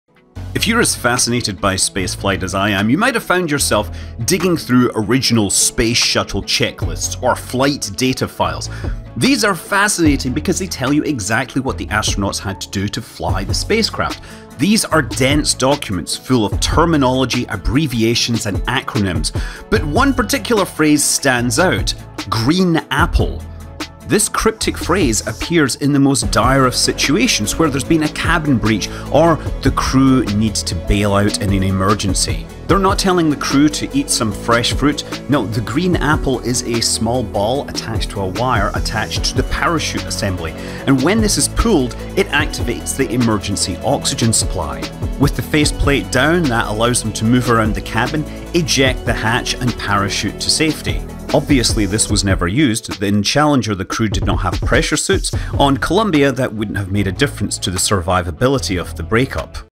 Why 'Green Apple' Was The Most Important Thing For Astronauts
What does a 'Green Apple' have to do with the Space Shuttle, and why was this cryptic term involved in so many critical checklists?